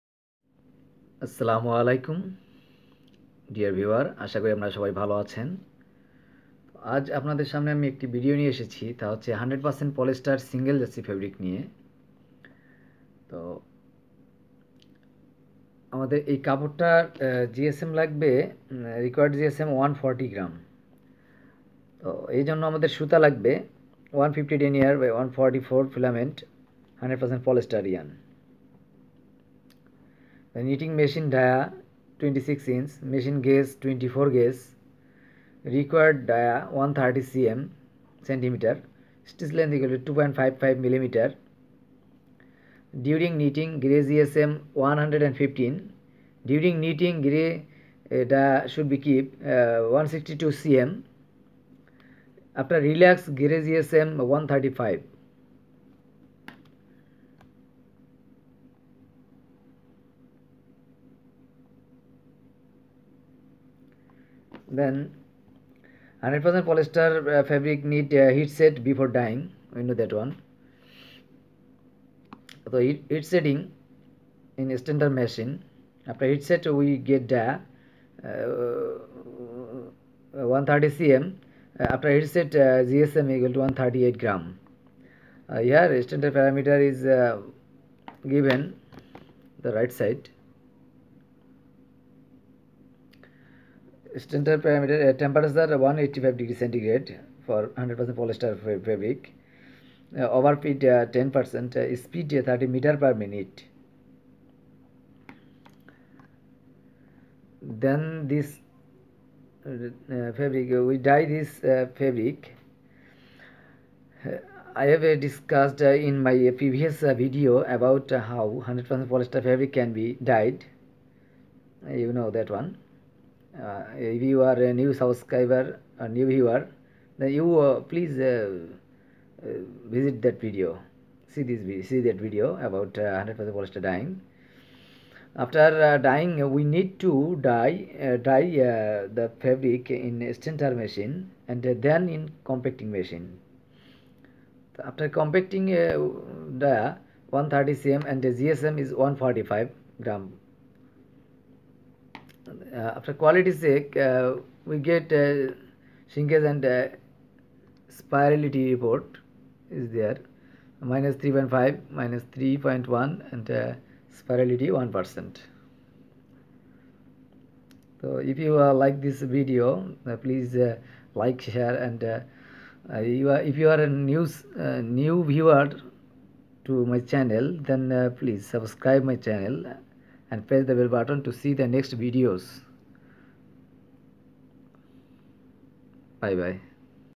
100% Polyester ll single Jersey fabric ll Knit fabric
This is a video about 100% polyester fabrics knowledge sharing abou knitting,  dyeing and finishing.